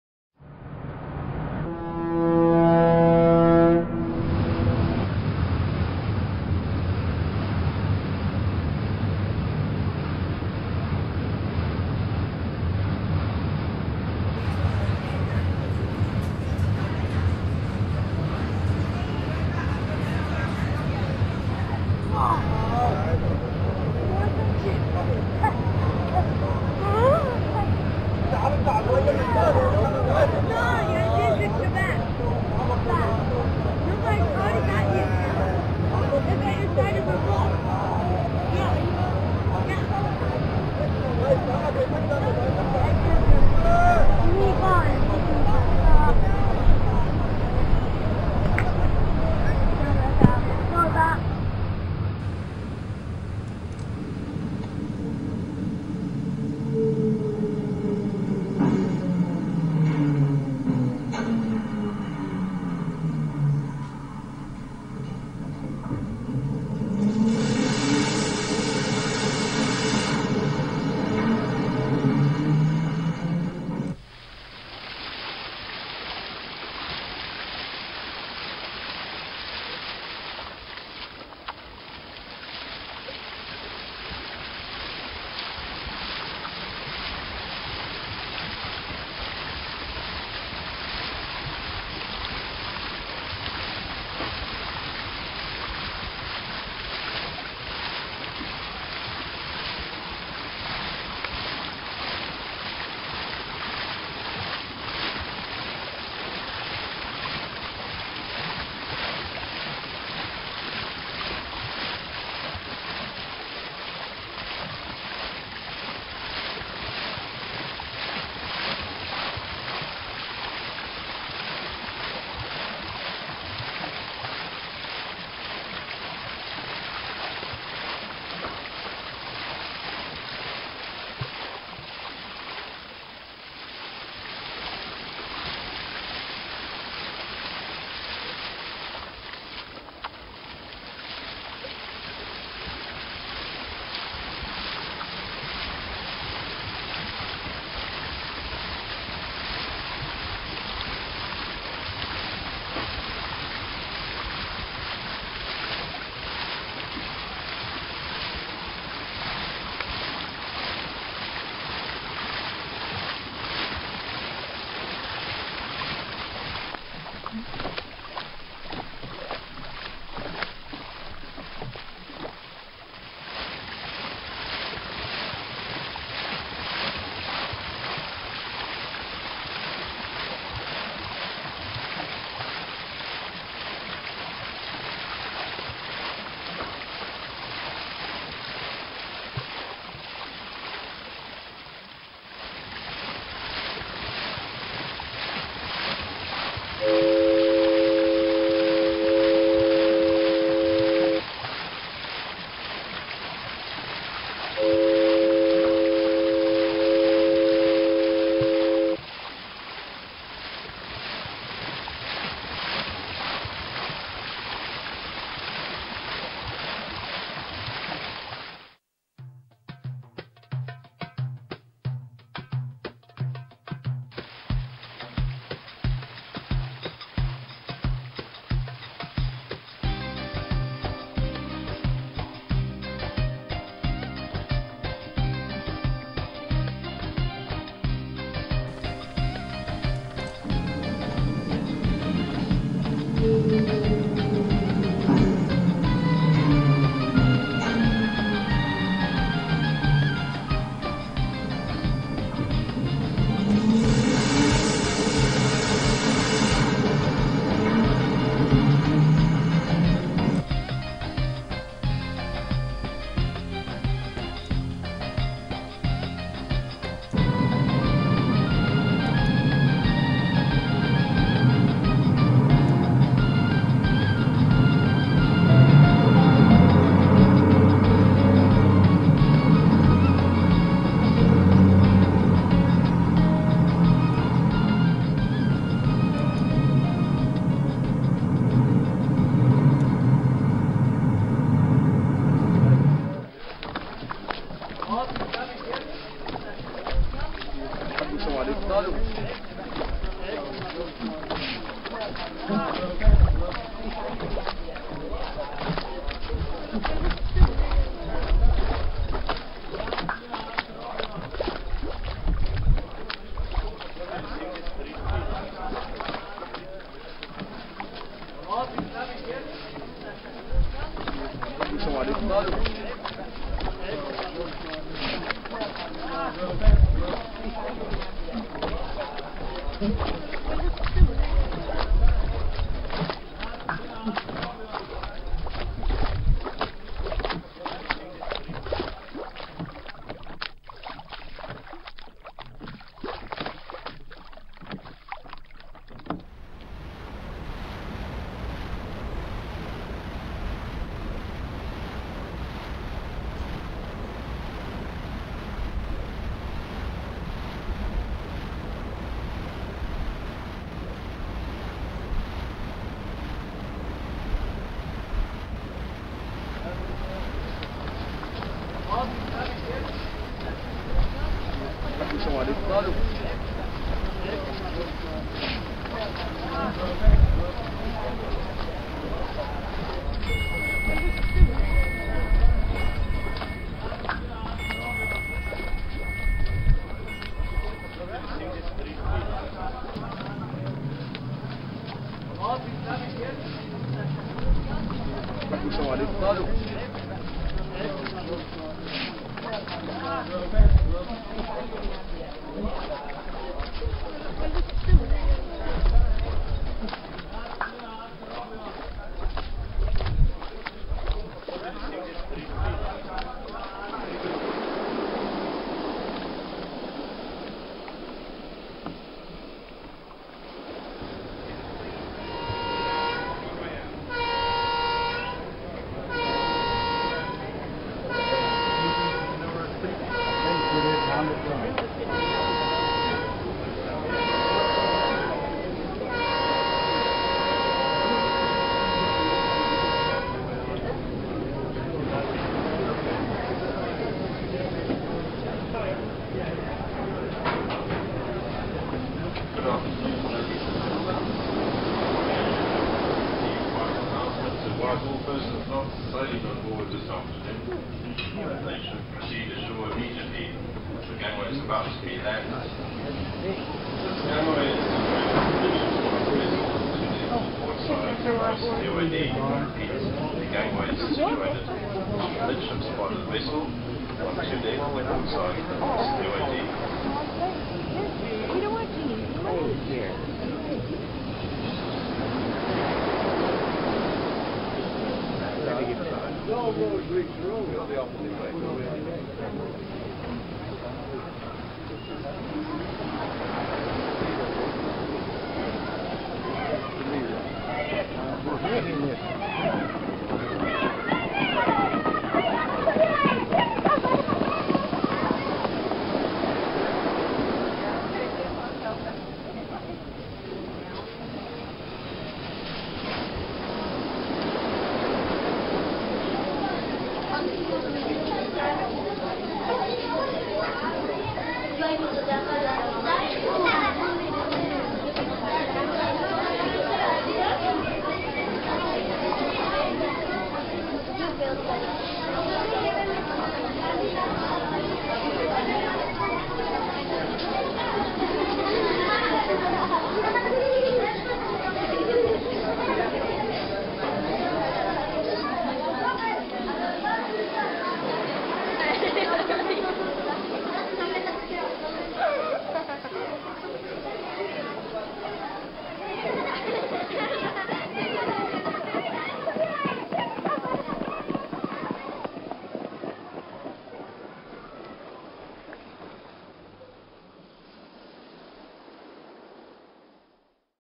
Cruise Singapore to Europe in 1950
ss Bayernstein leaves Singapore and sets sail to Europe through the Suex canal.Thimblerig is played with the children.